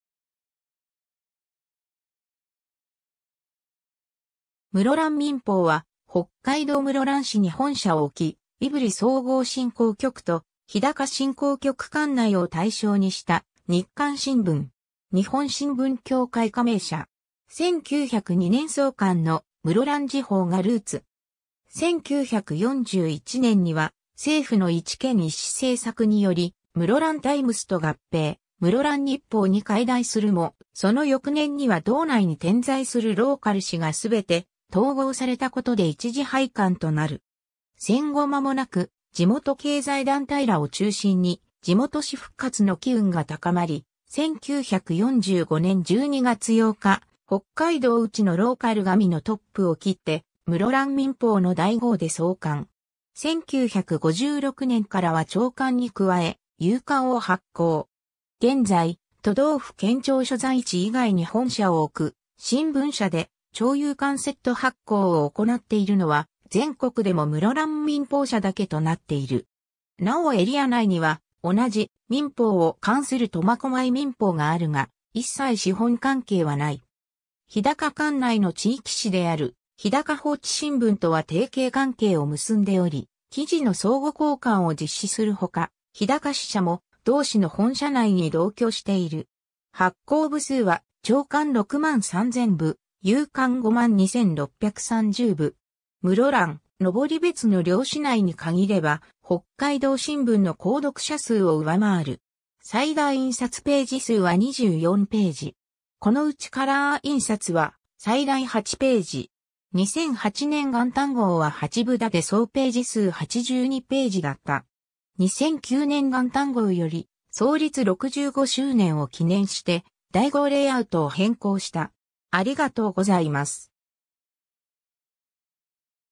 室蘭民報
室蘭民報（むろらんみんぽう、通称：むろみん）は、北海道室蘭市に本社を置き、胆振総合振興局と日高振興局管内を対象にした日刊新聞。
日本新聞協会加盟社。
 1902年創刊の「室蘭時報」がルーツ。
1941年には政府の一県一紙政策により室蘭タイムスと合併、「室蘭日報」に改題するも、その翌年には道内に点在するローカル紙が全て統合（当紙は北海道新聞社の室蘭支社となった）されたことで一時廃刊となる。
 戦後まもなく、地元経済団体らを中心に地元紙復活の機運が高まり、1945年12月8日北海道内のローカル紙のトップを切って「室蘭民報」の題号で創刊。
1956年からは朝刊に加え夕刊を発行。
現在、都道府県庁所在地以外に本社を置く新聞社で朝夕刊セット発行を行っているのは、全国でも室蘭民報社だけとなっている。
なおエリア内には同じ「民報」を冠する苫小牧民報があるが、一切資本関係はない。
日高管内の地域紙である日高報知新聞とは提携関係を結んでおり記事の相互交換を実施するほか、日高支社も同紙の本社内に同居している。
 発行部数は朝刊63,000部、夕刊52,630部（公称）。
室蘭・登別の両市内に限れば北海道新聞の購読者数を上回る（全国紙と室蘭民報を併読しているケースもある）。
最大印刷ページ数は24ページ。
このうちカラー印刷は最大8ページ。
2008年元旦号は8部建て総ページ数82ページだった。
2009年元旦号より、創立65周年を記念して題号レイアウトを変更した。